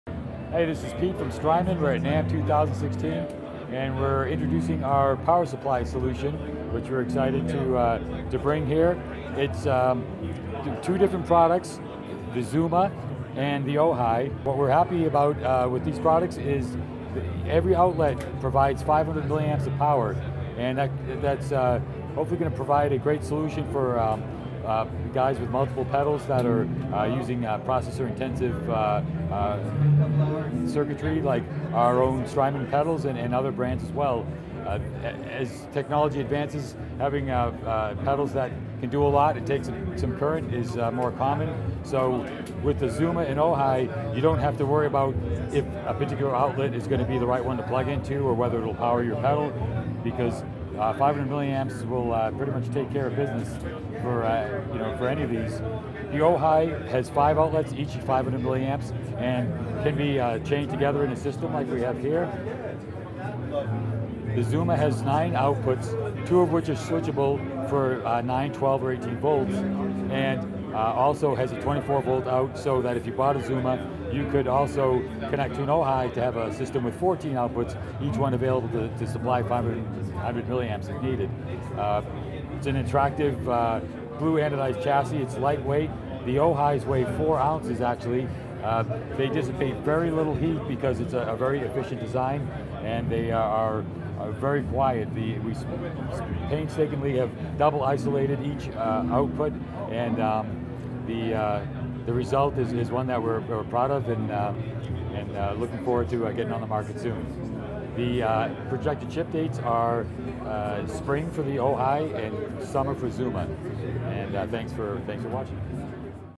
NAMM 2016 - Strymon Zuma & Ojai
Guitar World stops by the Strymon booth to check out some new gear at the 2016 winter NAMM show!